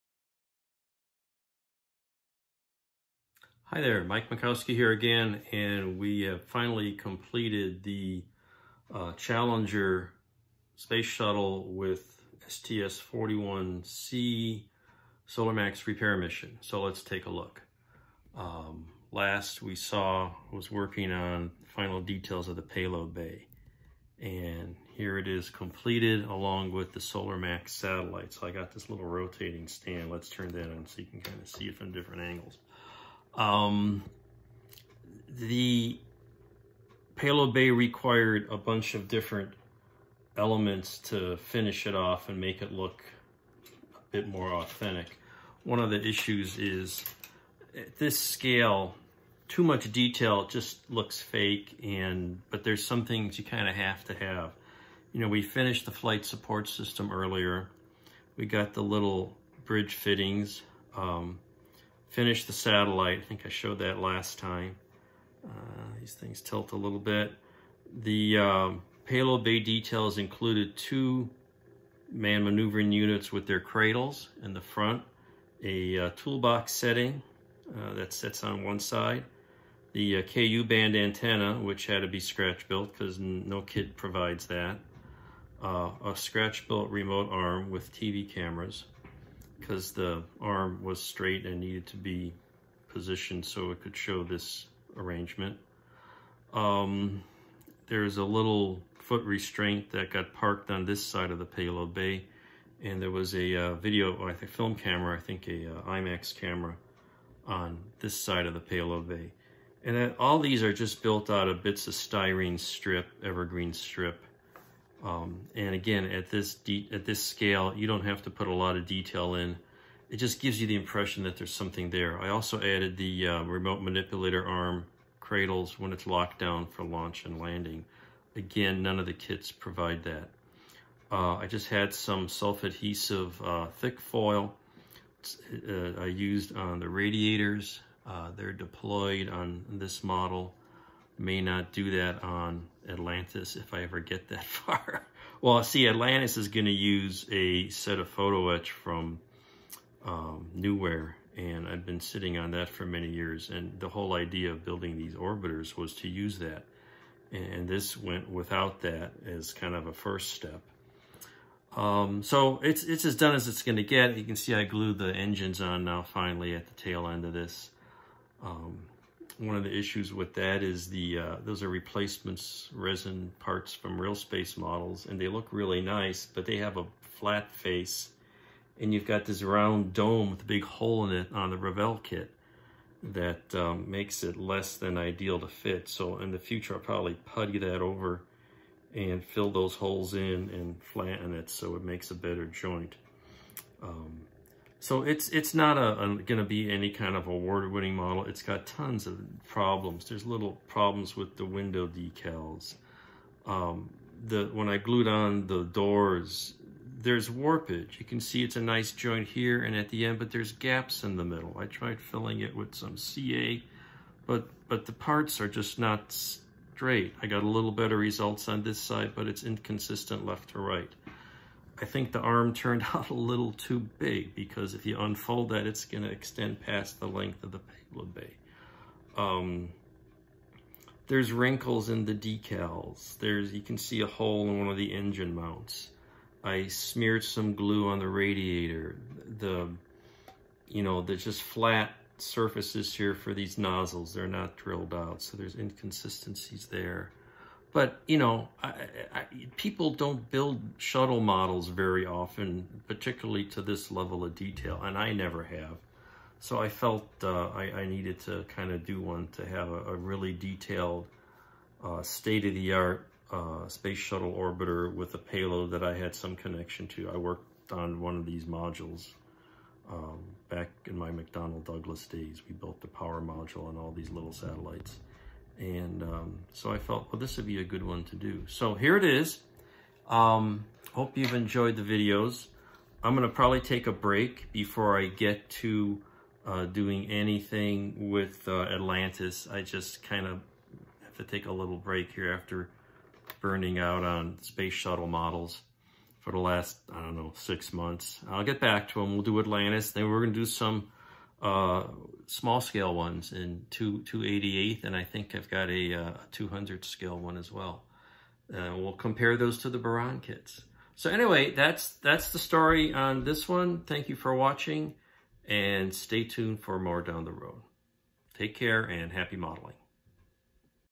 Shuttle 17 122620
The Challenger orbiter for STS-41C and the Solar Max repair mission is complete. I’ve always wanted to do a detailed Orbiter model in an on-orbit configuration. It has some issues as I point out in the video but considering the scale and the tiny details needed, it came out nice.